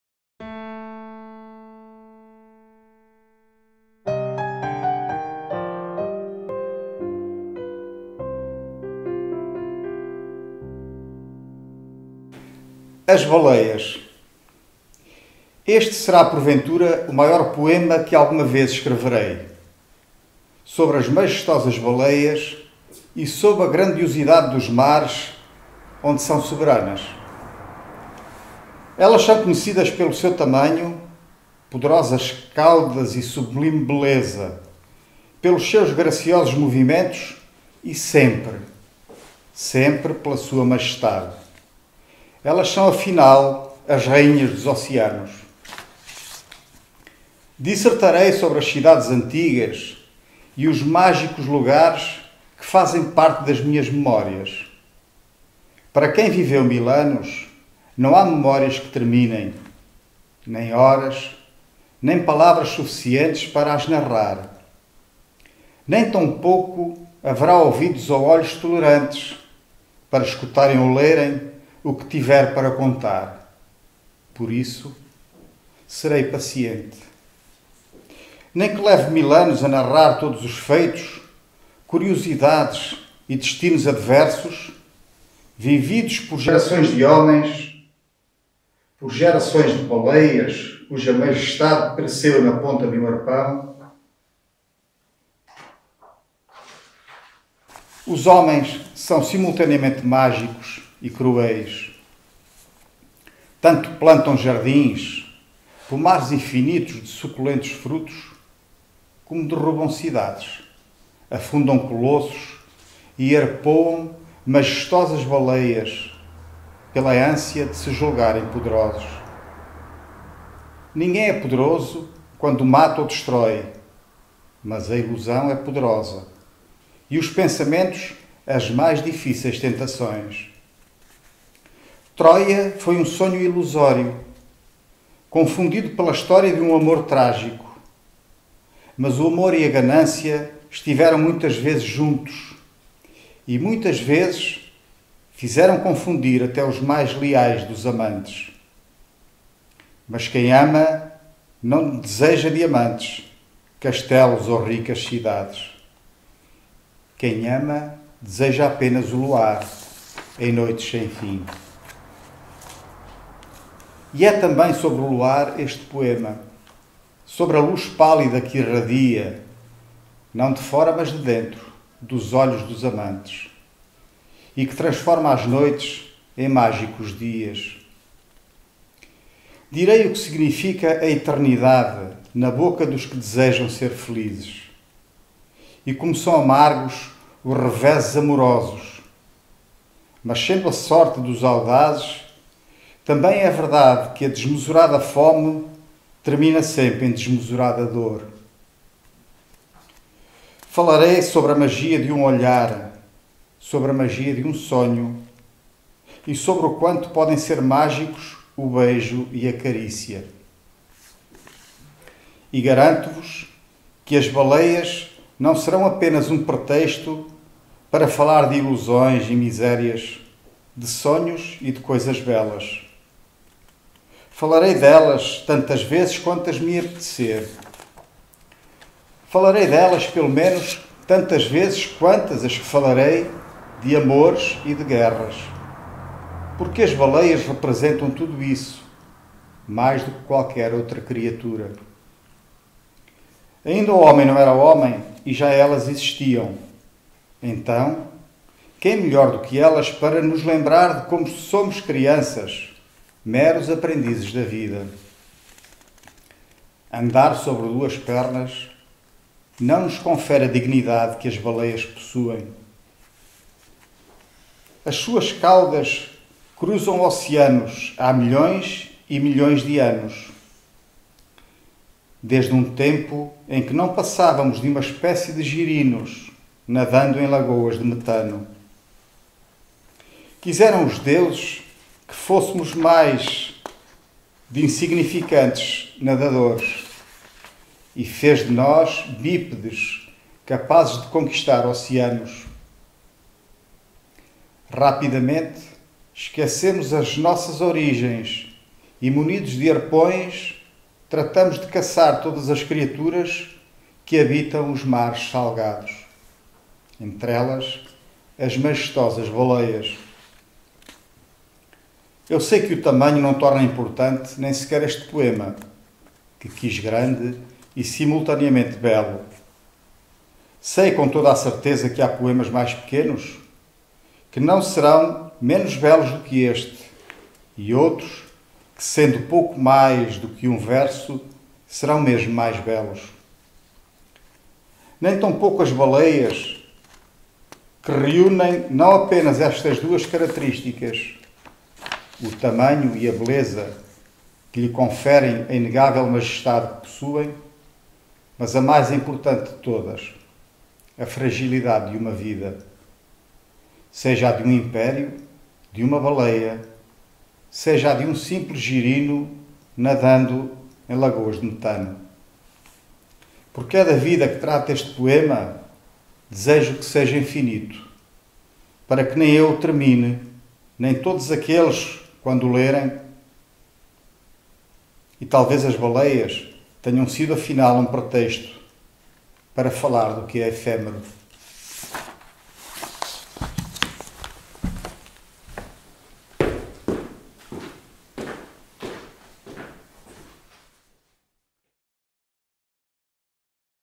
Poesia com Emílio Miranda 2
Emílio Miranda nasceu em Luanda, Angola, em 1966. Em 1975, vem viver para Lordelo, no norte do País, de onde os seus pais são originários.
Conta com uma dezena de livros de sua autoria.